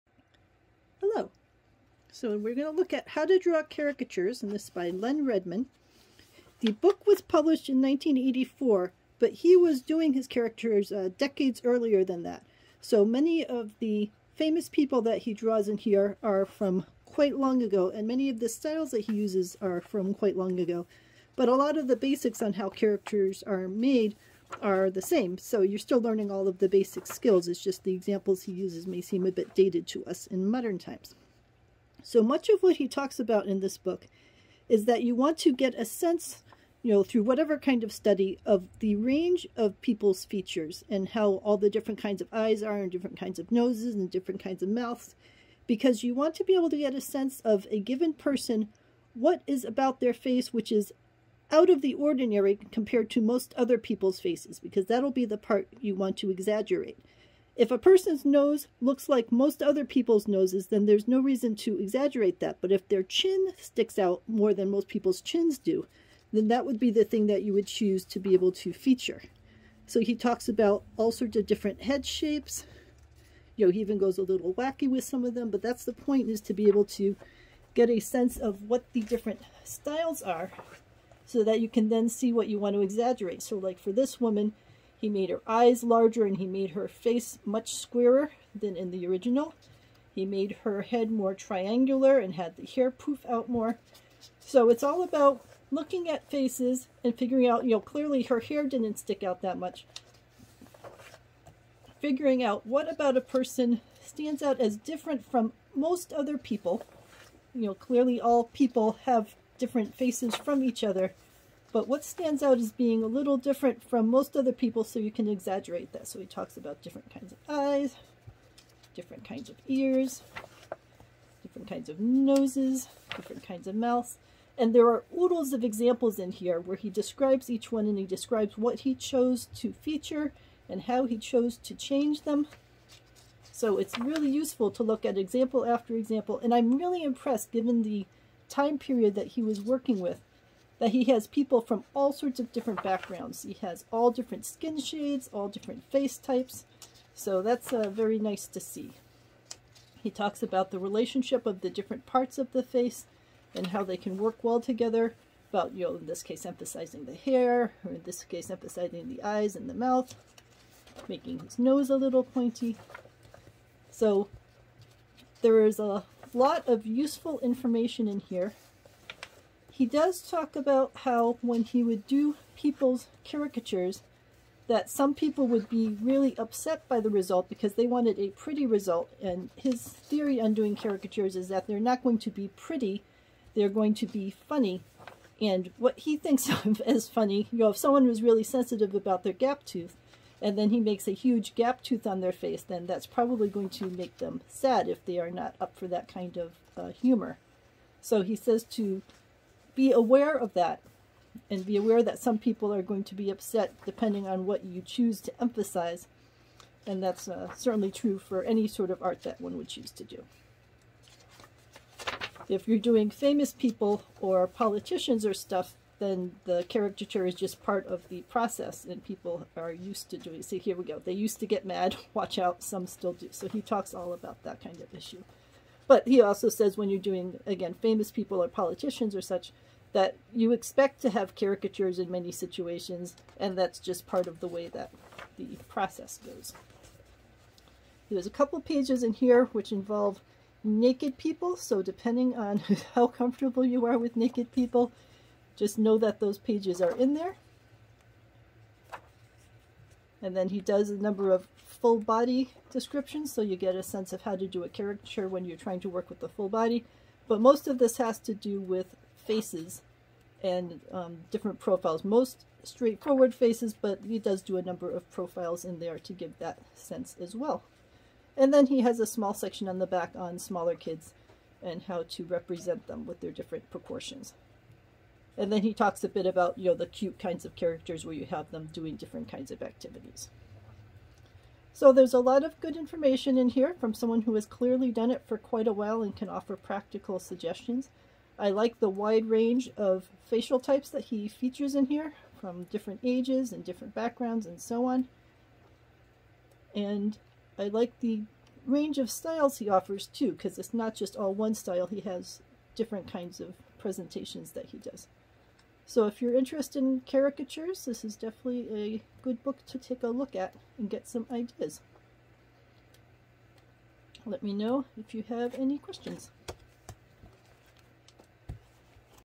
How To Draw Caricatures by Lenn Redman - Book Review
Lenn Redman is an experienced caricature artist who clearly has honed his talent over decades. While this book was published in 1984, much of the material is about actors, politicians, and styles from decades earlier than this. That's all right. The tips for doing caricatures remain the same.

Lenn assumes you already know the basics of sketching faces - there are plenty of books which step a beginner through that. He talks about selecting a pose, choosing which items to exaggerate, and developing your style. Most of the book is about the face, but he does touch on doing full-body caricatures and 'hobby based' drawings. He even has a few page of nude caricatures.

There are LOTS of facial examples from people of a wide variety of skin tones and styles, which I found quite refreshing in a book written in this older timeframe.

Well recommended.